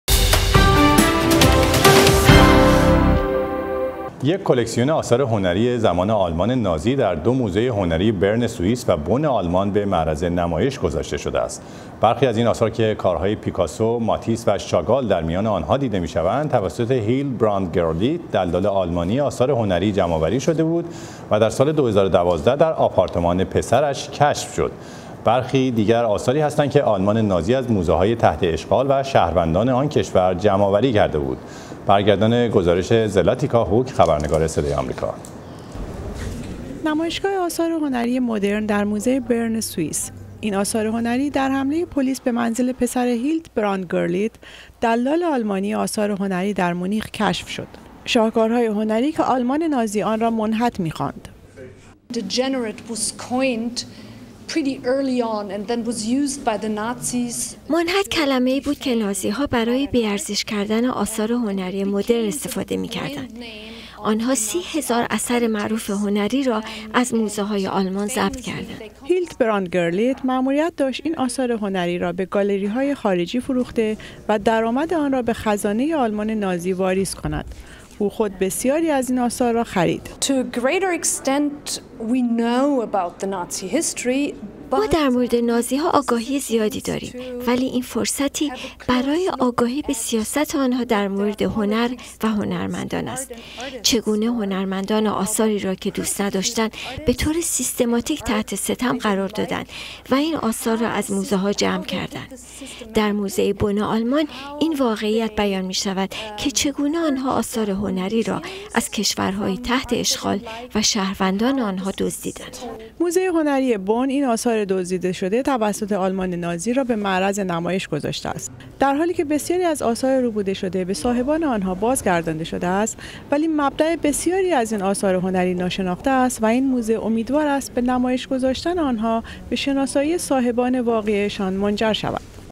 نمایشگاهی از آثار فاخر هنری که در زمان آلمان نازی قرار بود نابود شود
یک کلکسیون آثار هنری زمان آلمان نازی در دو موزه هنری برن سویس و بن آلمان به معرض نمایش گذاشته شده است.برخی از این آثار که کارهای پیکاسو ماتیس و شاگال در میان آن ها دیده می شوند توسط هیلدبراند گرلیت دلال آلمانیِ آثار هنری جمع آوری شده بود و در سال ۲۰۱۲ در آپارتمان پسرش کشف شد.